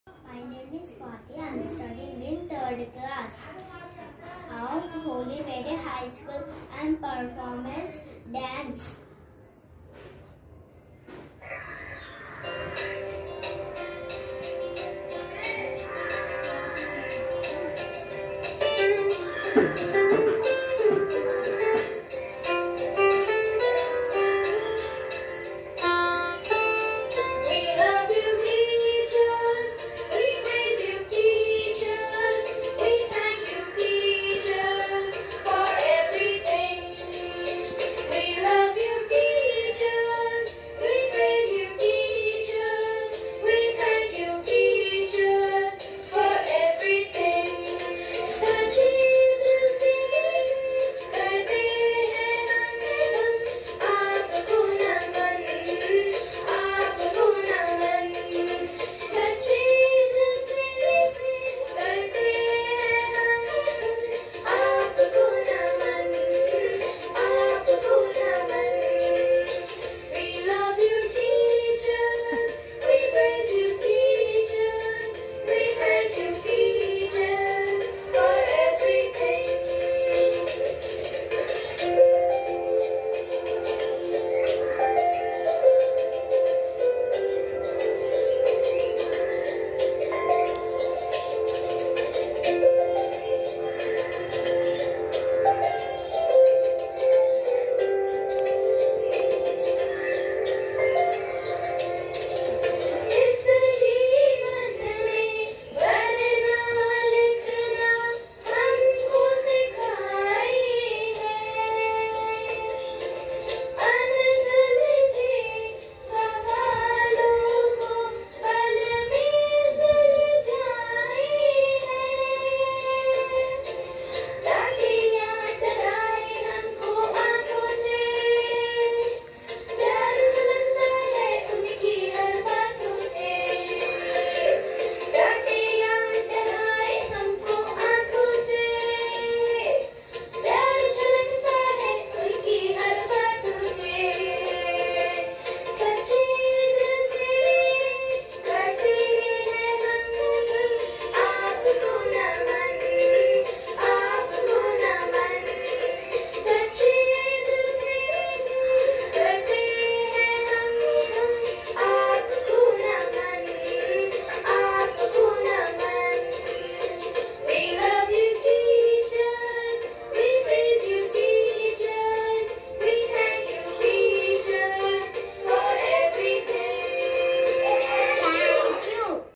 Teachers Day Dance and Song by G Swathi of Class 3 of Holy Mary High School Ferozguda Hyderabad 11
Video from Sukhdev Singh